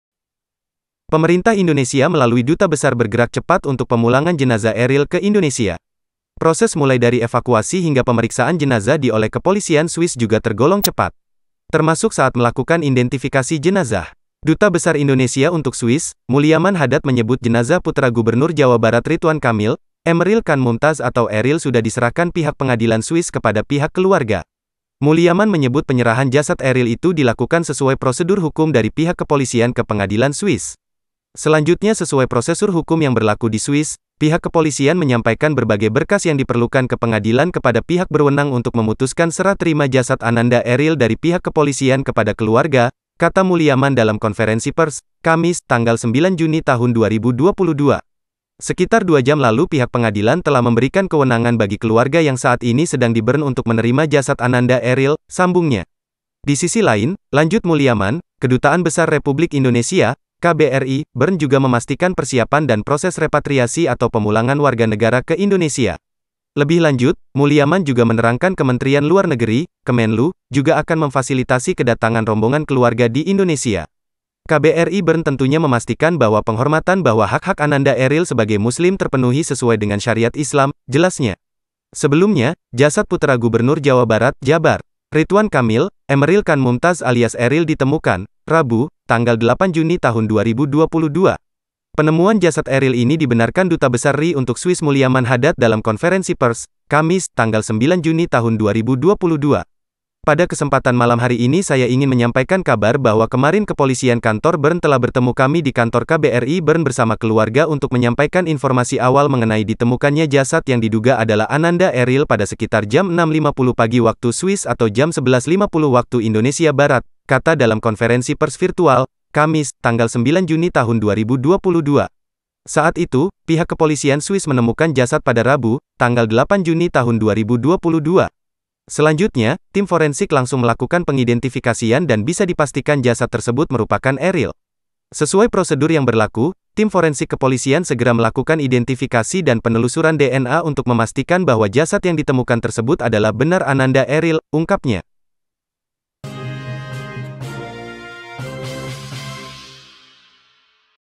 Prosedur Penyerahan Jenazah Eril Dari Pemerintah Swiss ke Indonesia, Kini Sudah di Pihak Keluarga
Pemerintah Indonesia melalui duta besar bergerak cepat untuk pemulangan jenazah Eril ke Indonesia.

Proses mulai dari evakuasi hingga pemeriksaan jenazah di oleh kepolisian Swiss juga tergolong cepat.

Termasuk saat melakukan indentifikasi jenazah.

Duta Besar Indonesia untuk Swiss, Muliaman Hadad menyebut jenazah putera Gubernur Jawa Barat Ridwan Kamil, Emmeril Kahn Mumtadz atau Eril sudah diserahkan pihak pengadilan Swiss kepada pihak keluarga.

Muliaman menyebut penyerahan jasad Eril itu dilakukan sesuai prosedur hukum dari pihak kepolisian ke pengadilan Swiss.

"Selanjutnya sesuai prosesur hukum yang berlaku di Swiss, pihak kepolisian menyampaikan berbagai berkas yang diperlukan ke pengadilan kepada pihak berwenang untuk memutuskan serah terima jasad ananda Eril dari pihak kepolisian kepada keluarga," kata Muliaman dalam konferensi pers, Kamis (9/6/2022).

"Sekitar 2 jam lalu pihak pengadilan telah memberikan kewenangan bagi keluarga yang saat ini sedang di Bern untuk menerima jasad ananda Eril," sambungnya.

Di sisi lain, lanjut Muliaman, Kedutaan Besar Republik Indonesia (KBRI) Bern juga memastikan persiapan dan proses repatriasi atau pemulangan warga negara ke Indonesia.

Lebih lanjut, Muliaman juga menerangkan Kementerian Luar Negeri (Kemenlu) juga akan memfasilitasi kedatangan rombongan keluarga di Indonesia.

"KBRI Bern tentunya memastikan bahwa penghormatan bahwa hak-hak ananda Eril sebagai muslim terpenuhi sesuai dengan syariat islam," jelasnya.

Sebelumnya, Jasad Putera Gubernur Jawa Barat (Jabar) Ridwan Kamil, Emmeril Kahn Mumtadz alias Eril ditemukan, Rabu (8/6/2022).

 Penemuan jasad Eril ini dibenarkan Duta Besar RI untuk Swiss Muliaman Hadad dalam konferensi pers, Kamis (9/6/2022).

"Pada kesempatan malam hari ini saya ingin menyampaikan kabar bahwa kemarin Kepolisian Kantor Bern telah bertemu kami di Kantor KBRI Bern bersama keluarga untuk menyampaikan informasi awal mengenai ditemukannya jasad yang diduga adalah ananda Eril pada sekitar jam 06.50 pagi waktu Swiss atau jam 11.50 Waktu Indonesia Barat," kata dalam konferensi pers virtual, Kamis (9/6/2022).

Saat itu, pihak kepolisian Swiss menemukan jasad pada Rabu (8/6/2022).

Selanjutnya, tim forensik langsung melakukan pengidentifikasian dan bisa dipastikan jasad tersebut merupakan Eril.

"Sesuai prosedur yang berlaku, tim forensik kepolisian segera melakukan identifikasi dan penelusuran DNA untuk memastikan bahwa jasad yang ditemukan tersebut adalah benar ananda Eril," ungkapnya.